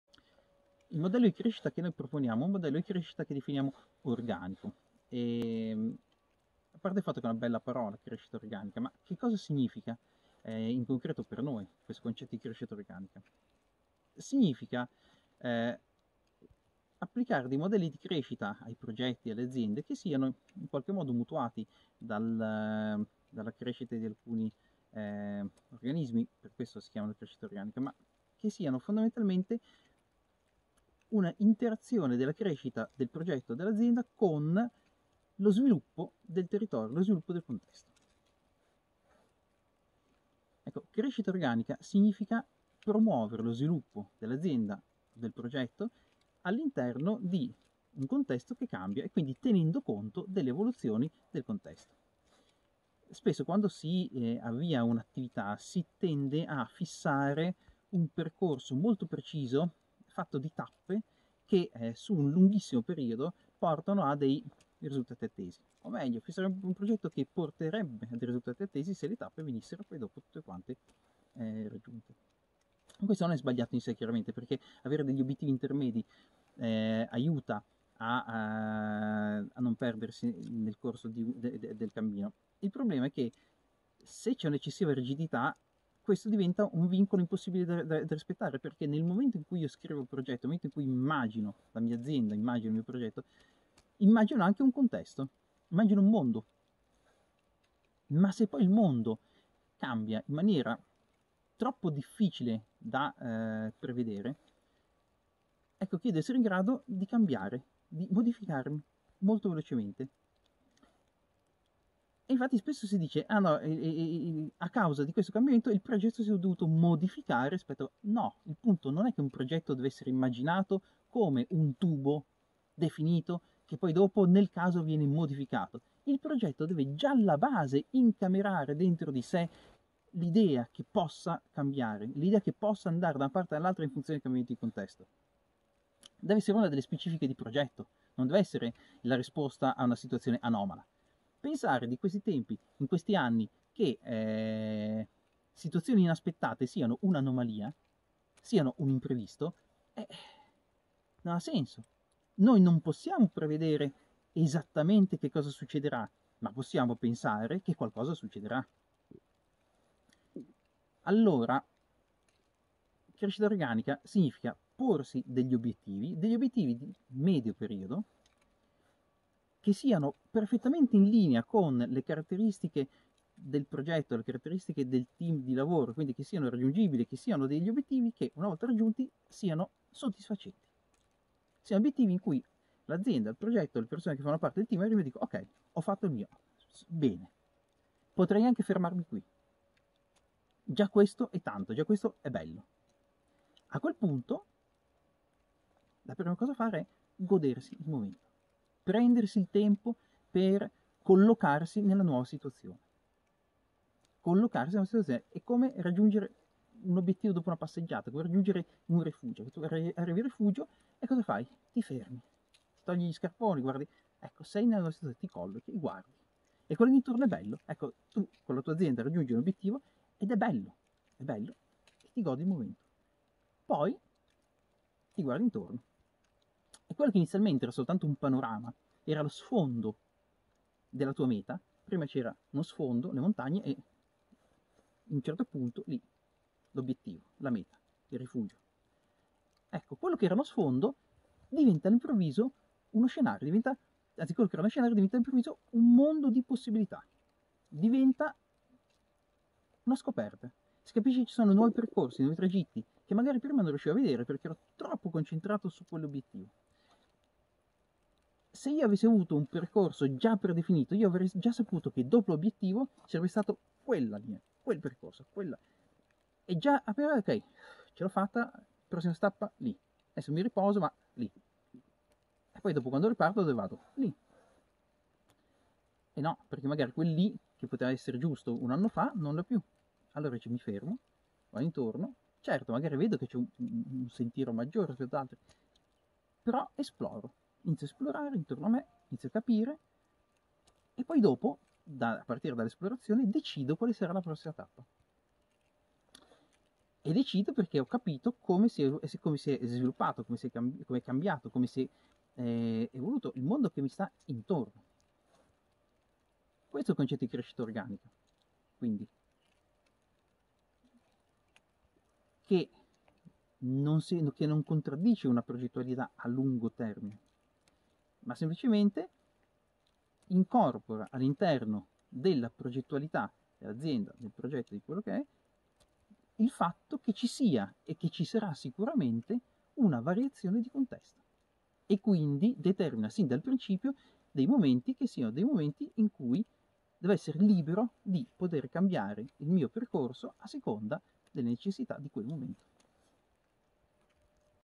Dalle Idee all'Impresa con una Crescita Organica // LIBERTA' OBBLIGATORIA
Il modello di crescita che proponiamo è un modello di crescita organico.
Un'interazione della crescita dell'azienda con lo sviluppo del territorio.
Crescita organica significa promuovere lo sviluppo dell’azienda all’interno di un contesto che cambia tenendo conto delle evoluzioni del contesto.
Un’azienda che cresce in maniera organica sarà per sua natura capace di adattarsi e di modificarsi e quindi sarà più idonea ad affrontare il cambiamento.
Crescita organica significa porsi alcuni obiettivi intermedi concreti e facilmente raggiungibili.
Una volta raggiunti questi obiettivi ci si ferma, si gode di ciò che si trova e ci si prende il tempo per esplorare che cosa c’è intorno in modo tale dal poter individuare nuovi obiettivi e scegliere nuove direzioni. Tantissimi esseri viventi operano in questo modo e possono farlo anche le aziende.

In un’azienda il cambiamento di direzione non deve essere una risposta anomala ad un imprevisto. Crescita organica significa porsi degli obiettivi di medio periodo che una volta raggiunti siano soddisfacenti.
A quel punto bisogna prendersi del tempo per collocarsi nella nuova situazione per
1. godersi il momento
2. guardarsi intorno per esplorare le diverse nuove opportunità e decidere quale sarà il prossimo obiettivo.